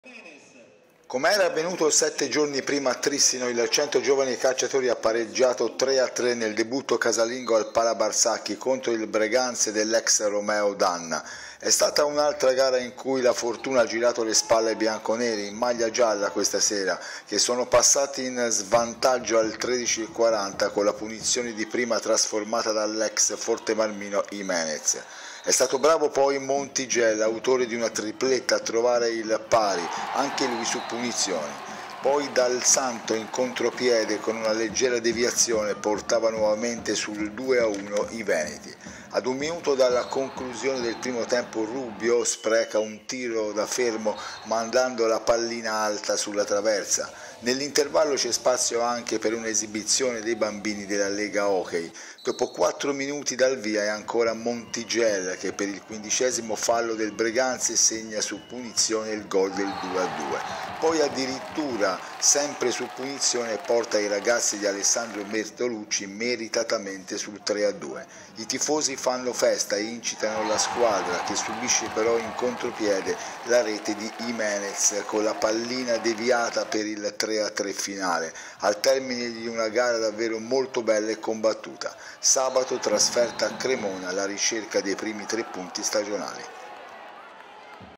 Ancora un pari per il CGC: 3-3 contro Breganze
HOCKEY A1 - Finisce 3 a 3 il bigmatch della seconda giornata tra CGC Viareggio e Breganze. Gara d'orgoglio dei bianconeri che rimontano da 1-3 grazie ad uno scatenato Montigel.

NoiTv